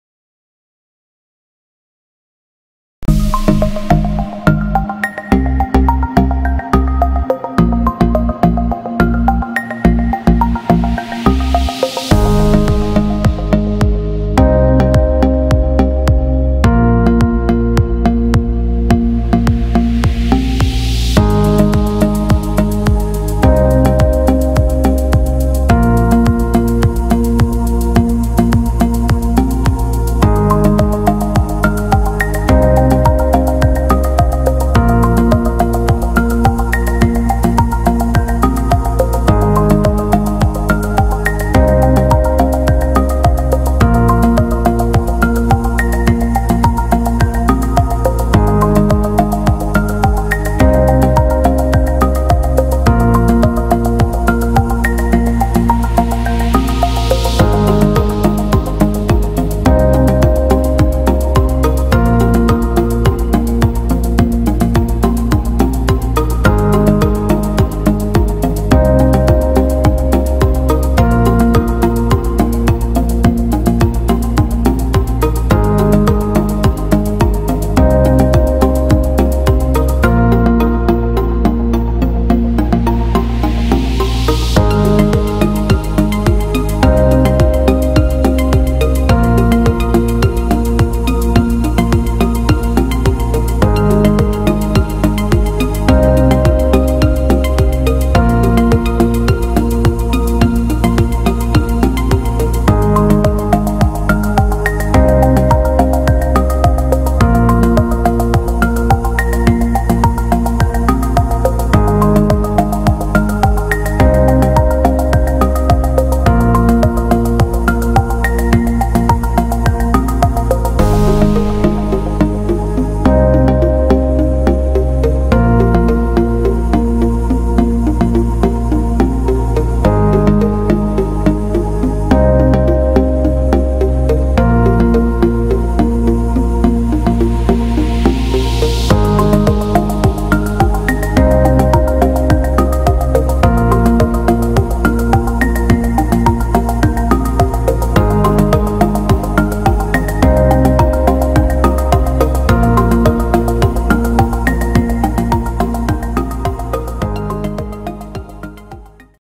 Atardeceres El Salvador Drone Mavic 2 Zoom
Vistas de Atardeceres en diferentes partes de nuestro país El Salvador, toda la fotografía hecha por Drone. Para recordar que no importa donde estés tu país siempre esperará por ti.